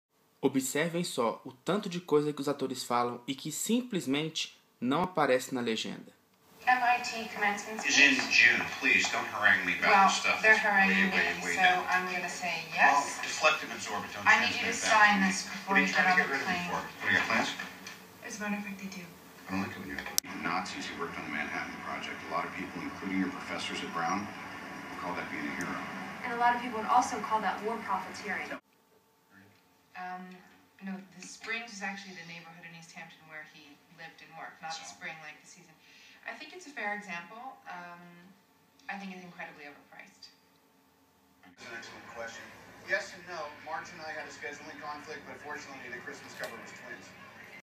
Como é ruim ver filme legendado
Um pouco da prova de que assistir filme legendado é muito pior do que ver dublado, para quem não domina o inglês. Procuro evidenciar as partes não legendadas, mas há muitos outros problemas em ver filmes legendados, como a perda do enquadramento do filme, a perda do entendimento de expressões irônicas, o olhar muito na parte de baixo da tela que atrapalha ver a atuação dos atores e por aí vai. Se não entende inglês, melhor ver dublado.